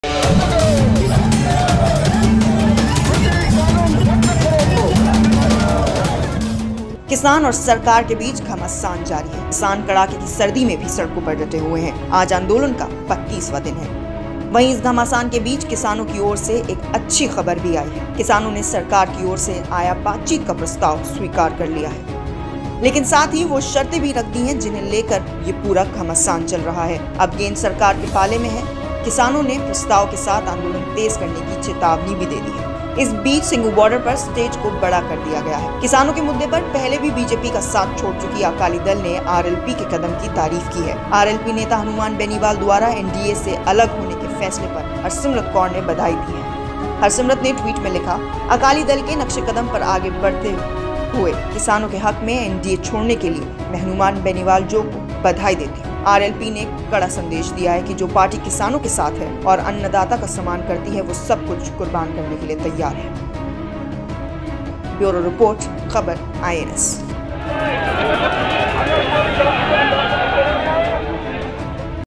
हरसिमरत ने हनुमान बेनीवाल को दी बधाई/Harsimrat congratulates Hanuman Beniwal
किसान और सरकार के बीच घमासान जारी है. किसान कड़ाके की सर्दी में भी सड़कों पर डटे हुए हैं. आज आंदोलन का 32वां दिन है. वहीं, इस घमासान के बीच किसानों की ओर से एक अच्छी खबर भी आई है. किसानों ने सरकार की ओर से आया बातचीत का प्रस्ताव स्वीकार कर लिया है. लेकिन साथ ही वो शर्तें भी रख दी हैं, जिन्हें लेकर ये पूरा घमासान चल रहा है. अब गेंद सरकार के पाले में है. किसानों ने प्रस्ताव के साथ आंदोलन तेज करनी की चेतावनी भी दे दी है. इस बीच सिंघु बॉर्डर पर स्टेज को बड़ा कर दिया गया है.